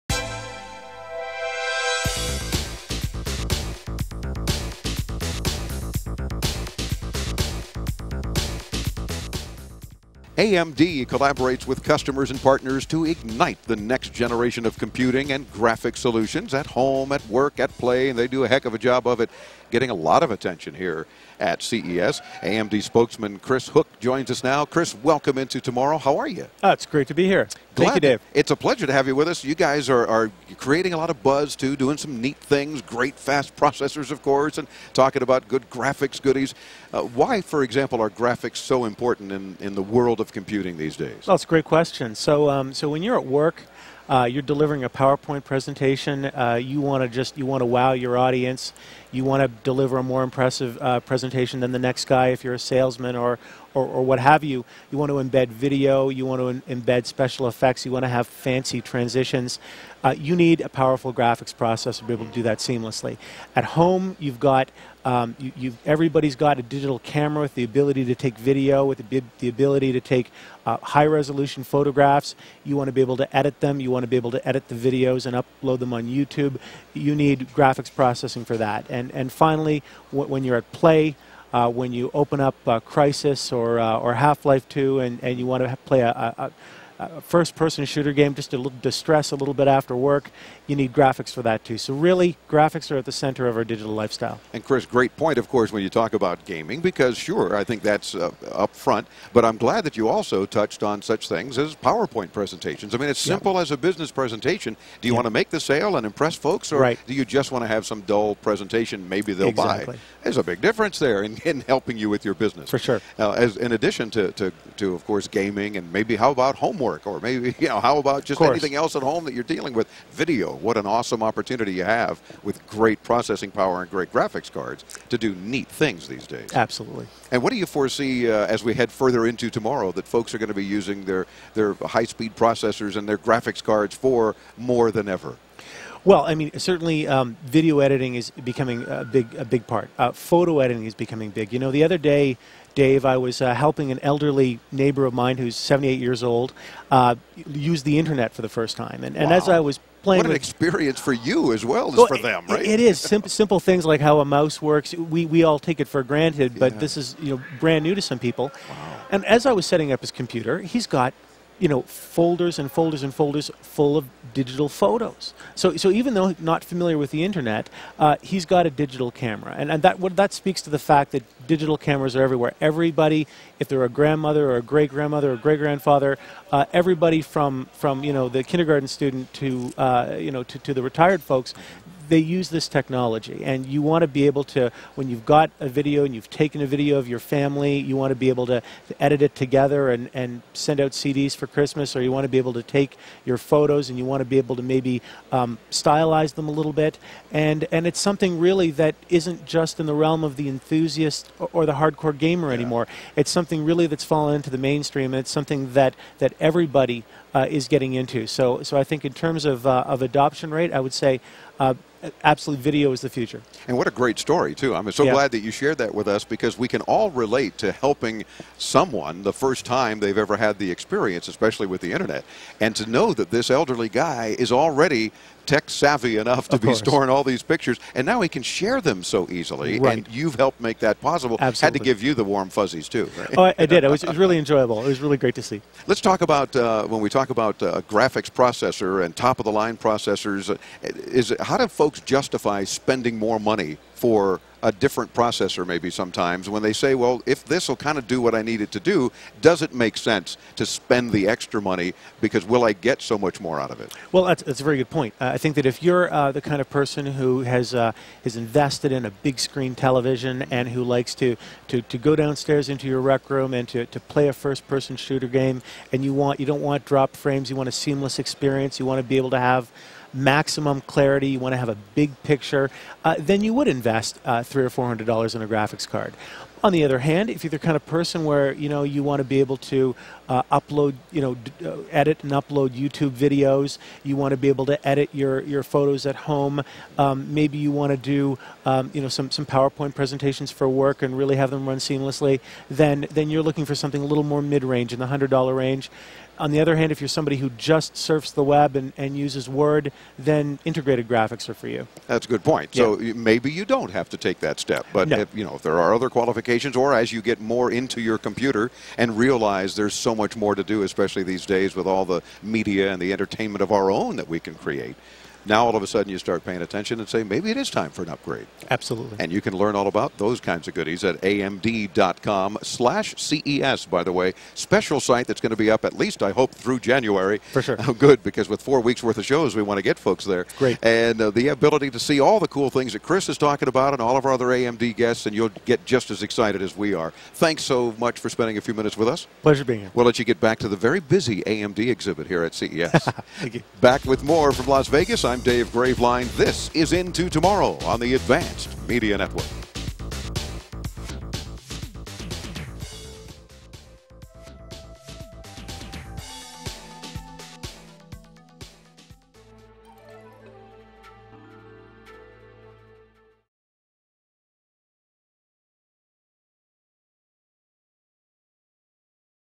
CES 2009: AMD
Dave Graveline interviews Chris Hook from AMD at the 2009 International Consumer Electronic Show (CES) in Las Vegas. Into Tomorrow celebrates 14 years ON-THE-AIR.